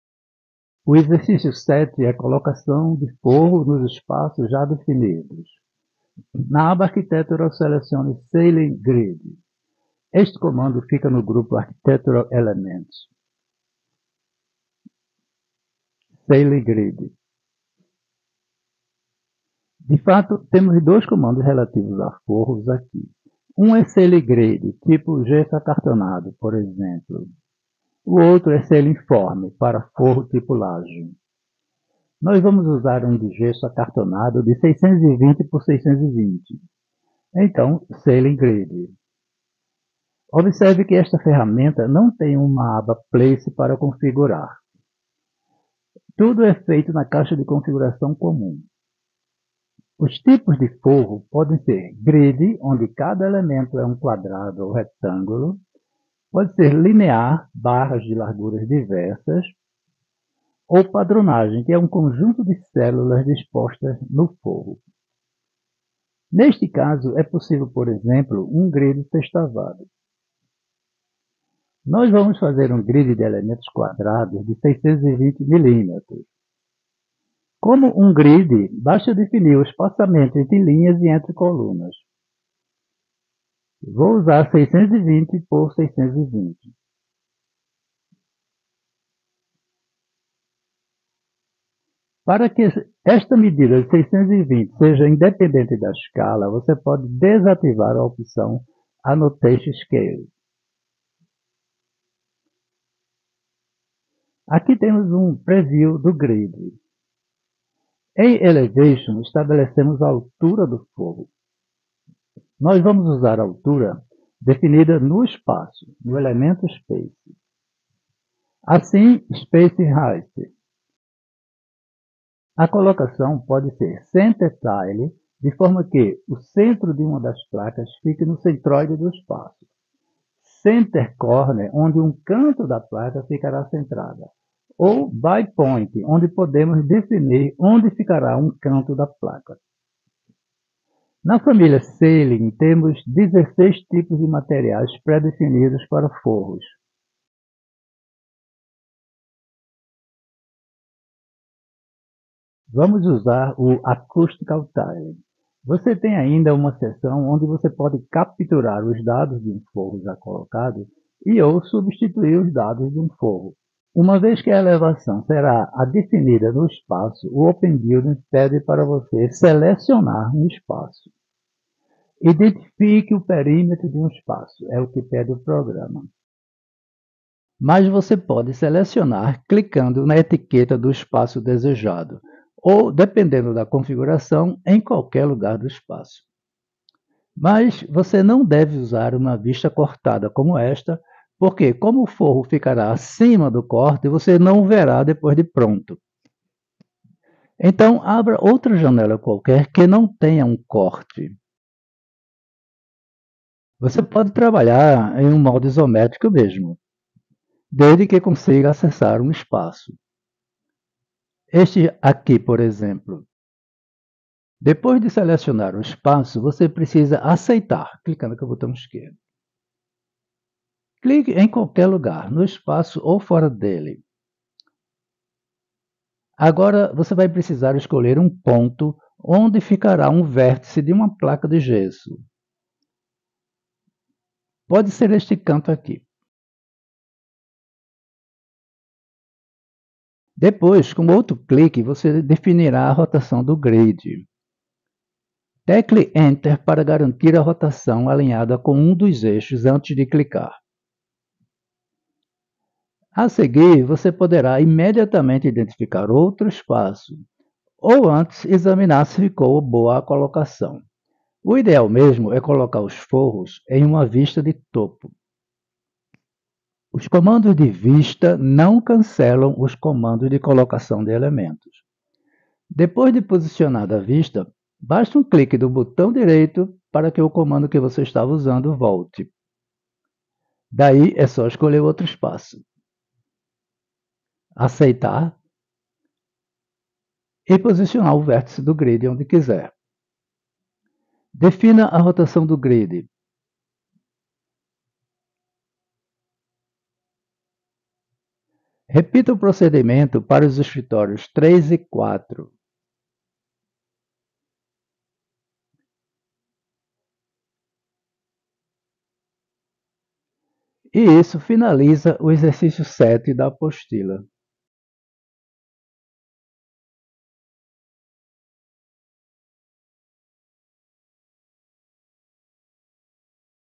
Ex 7 Forro Ceilings Apostila A102 Modelando Pavimento
Neste vídeo a colocação forro (Ceilings).